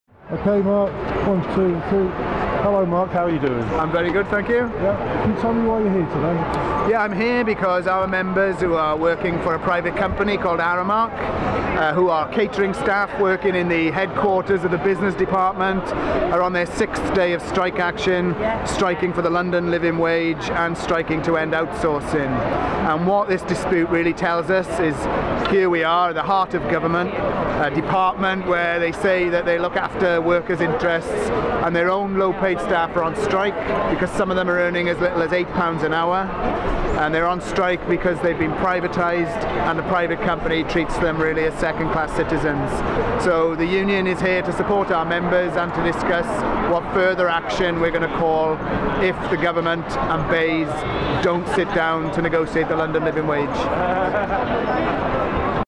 Public and Commercial Services Union
3-day BEIS catering strike rounds off with strongly supported picket Mark Sewotka pcs
A well-supported picket line, backed by a visit from shadow business secretary Rebecca Long Bailey, rounded off a 3-day pay strike by PCS members who are contracted catering staff working at the Department for Business, Energy and Industrial Strategy.